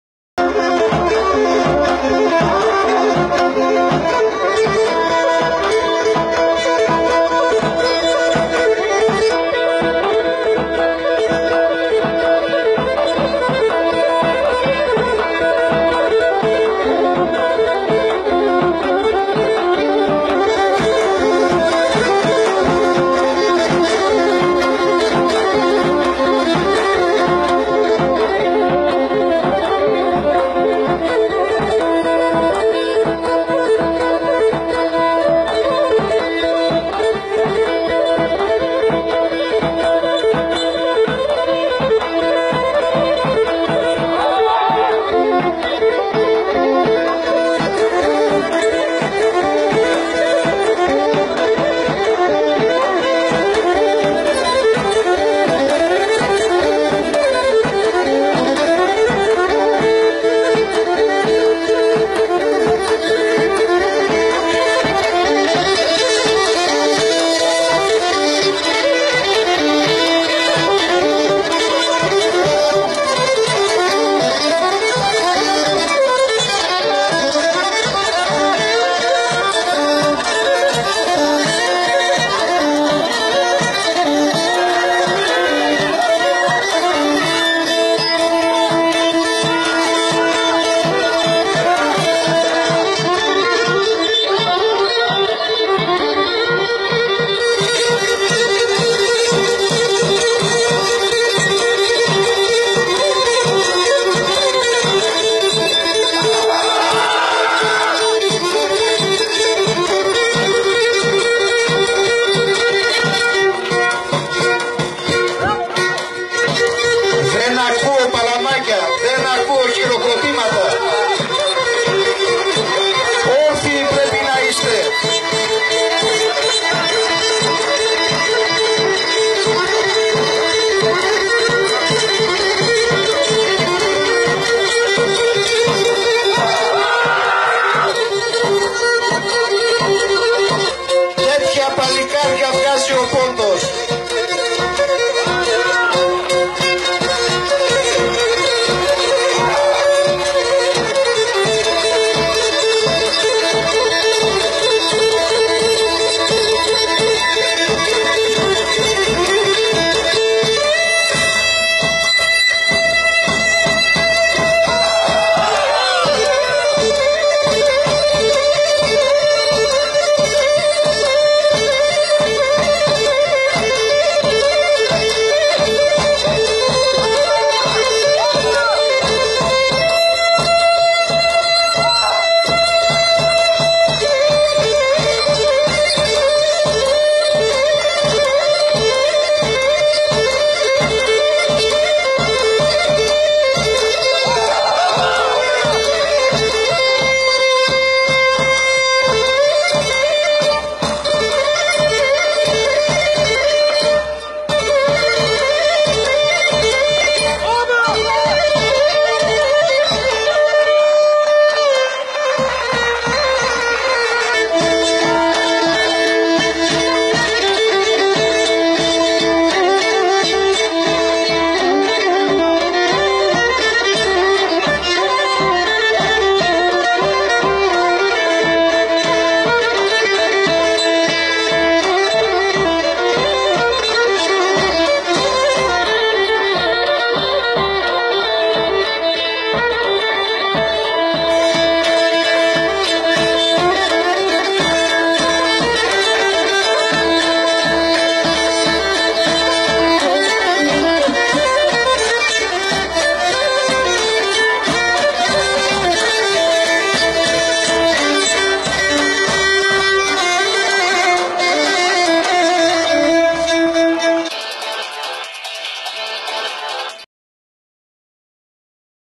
ΛΕΤΣΙΝΑ & ΣΕΡΡΑ - ΕΚΔΗΛΩΣΕΙΣ ΠΡΟΦΗΤΗ ΗΛΙΑ ΑΓΡΟΣΥΚΙΑΣ (Νο1)
ΒΙΝΤΕΟ Αντώνης Παπαδόπουλος (ΣΥΜΒΑΙΝΕΙ ΤΩΡΑ) 20/7/2017 - Διοργάνωση: Σωματείο "ΑΓΙΟΣ ΓΕΩΡΓΙΟΣ ΧΑΛΙΝΑΡΑΣ ΧΑΡΣΕΡΑΣ ΑΡΓΥΡΟΥΠΟΛΕΩΣ ΤΟΥ ΠΟΝΤΟΥ - Στον Λόφο του Προφήτη Ηλία Αγροσυκιάς.